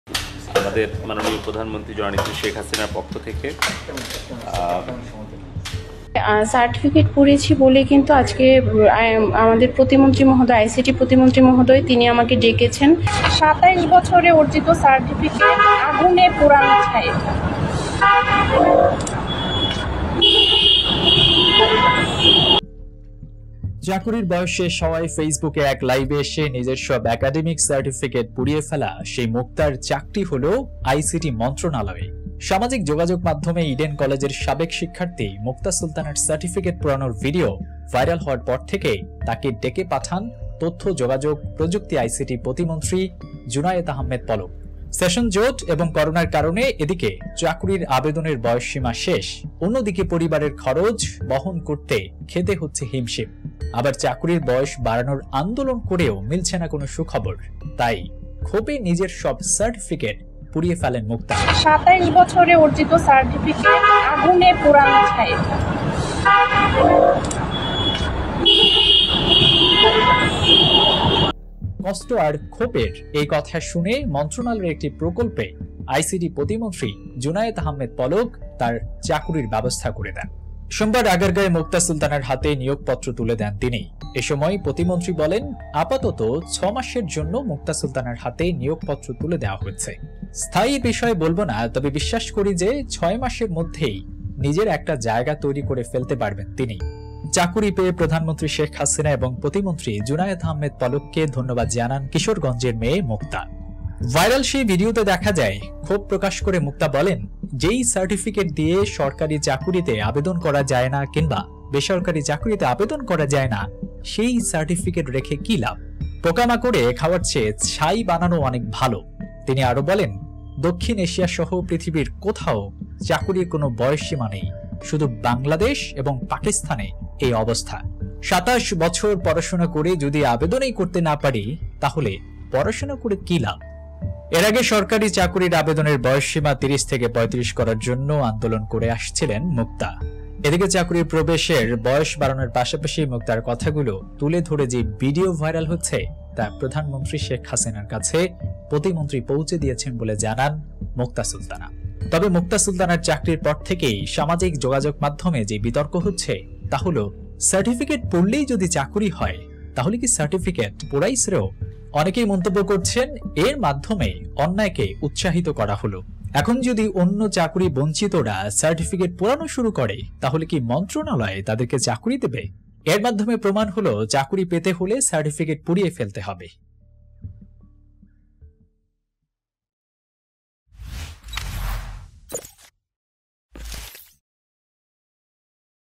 ভাইরাল তরুনীর চাকুরির খবরে দেশে সার্টিফিকেট পুড়ানোর হিড়িক ! Mukta Sultana update | Breaking news
চাকরি পেল সার্টিফিকেট পুড়িয়ে ফেলা তরুণী